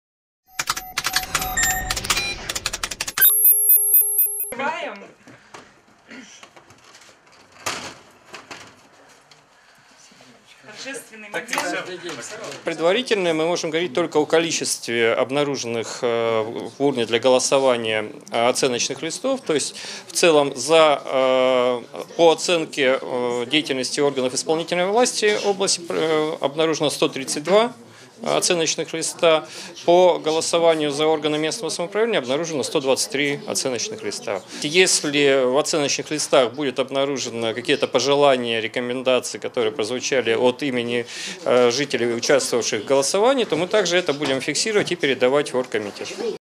03.03.2014 Команда губернатора: Ваша оценка
В Вологодской области подсчитывают голоса проекта «Команда губернатора: Ваша оценка».